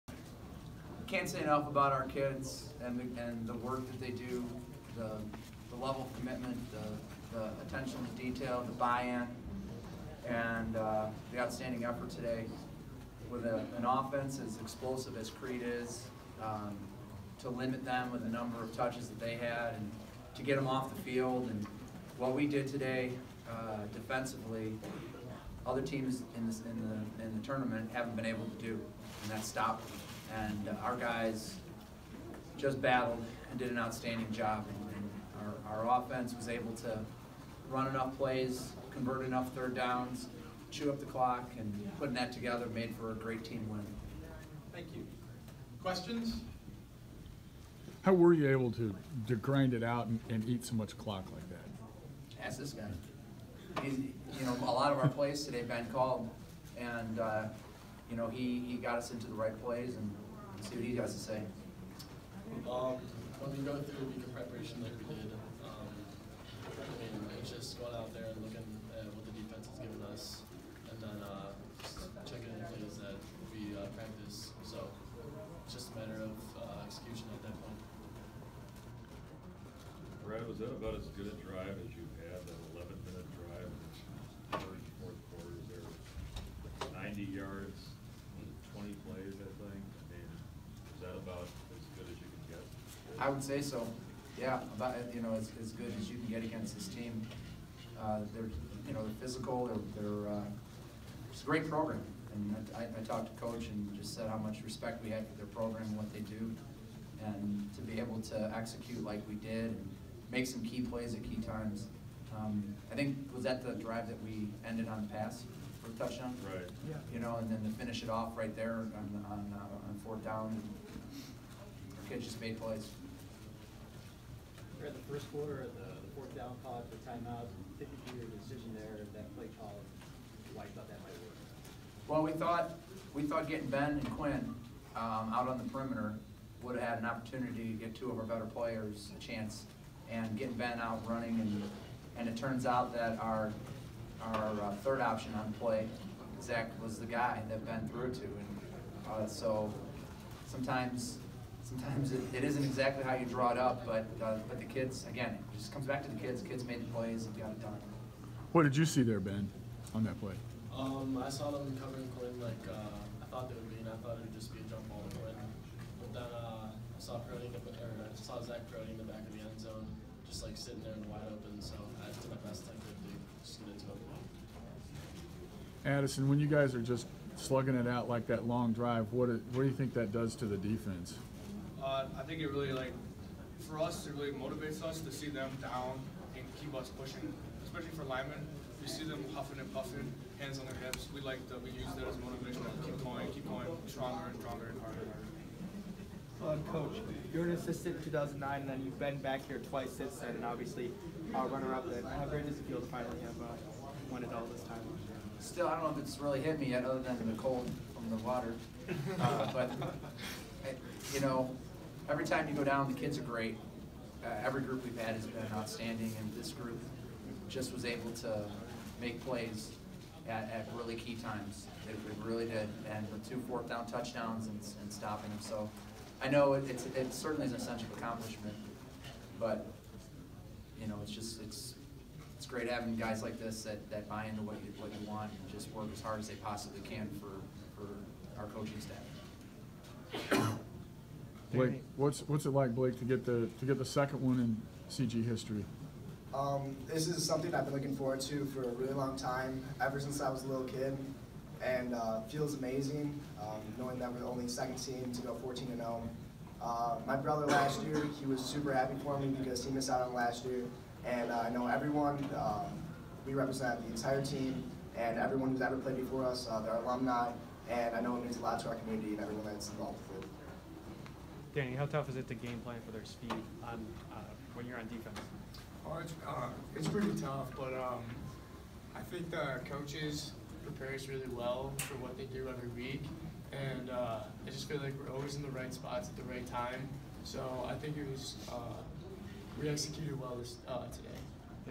Cary-Grove wins 6A state title
From left: C-G’s Addison West, Blake Skol, coach Brad Seaburg, Ben McDonald and Danny Daigle talk about their win over Crete-Monee.